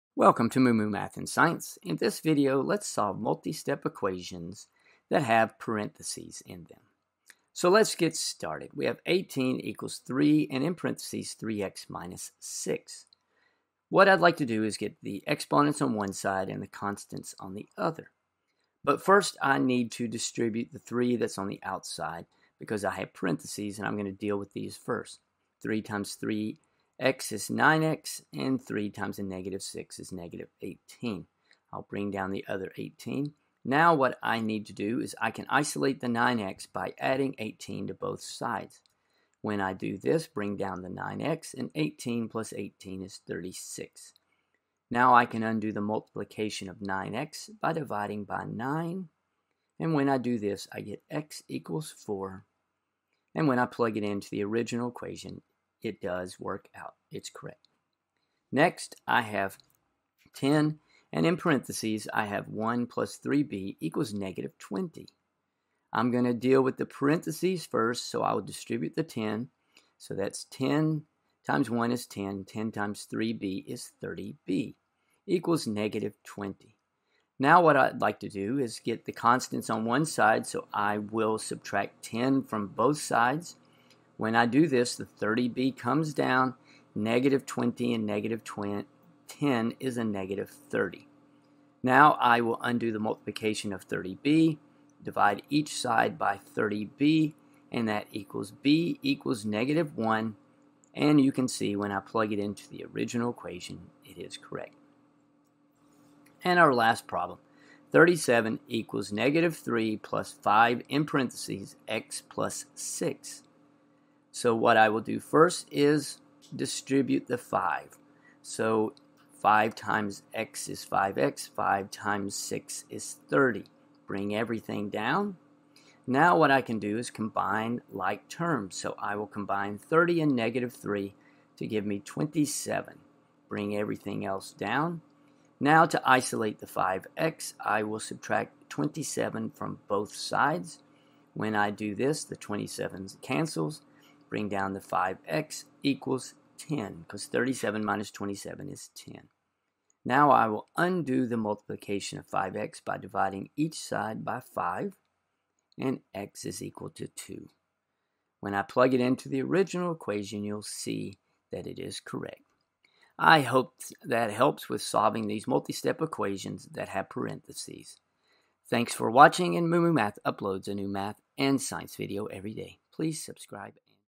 Solve Multi-step Equations
Learn how to solve multi-step equations. These equations contain parenthesis. The key to these multi-step equations is to get variables on one side of the equation and the constants on the other.
In this video I solved the following problems.
18 = 3(3x − 6)
37 = −3 + 5(x + 6)
10(1 + 3b) = −20